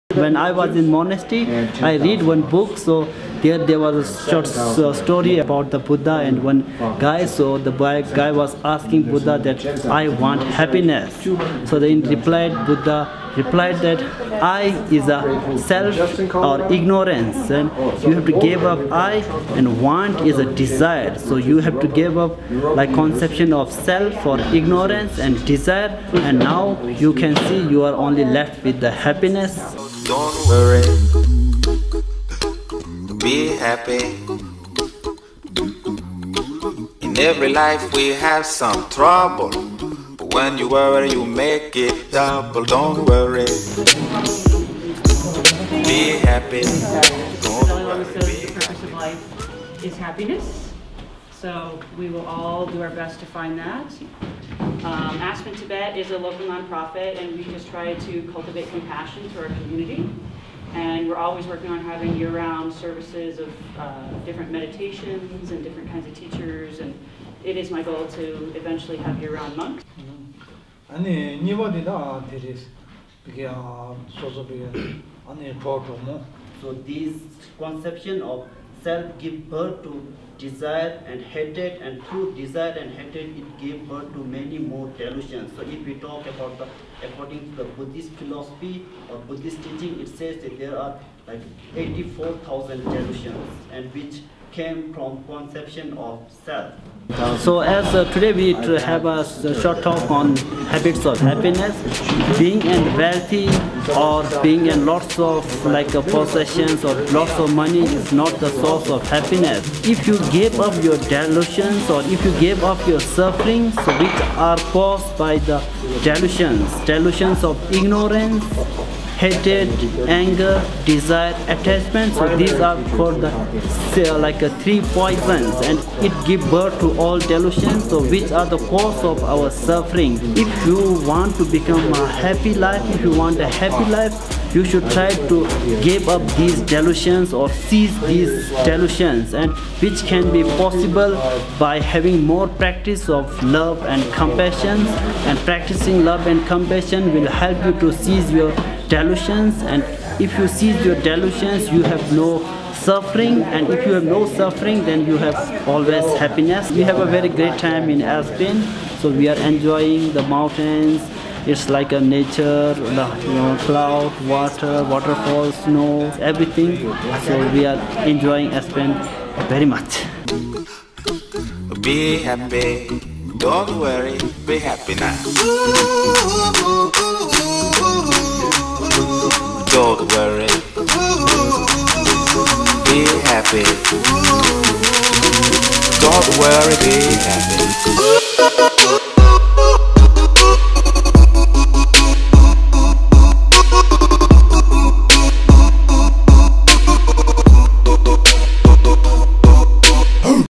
HABITS OF HAPPINESS
Spent a day Learning from Aspen Tibet about how to live happier.